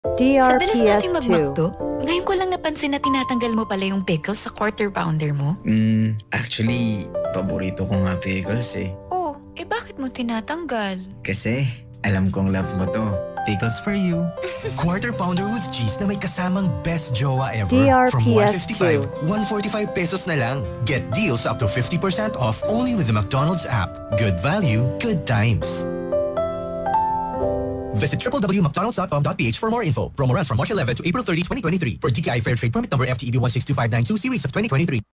McDonald's App Radio Ad Q1 2023 35s
Ito po ang Q1 2023 Radio Ad ng McDonald's App.

DRPS Two 3674: McDonald's App Radio Ad Q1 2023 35s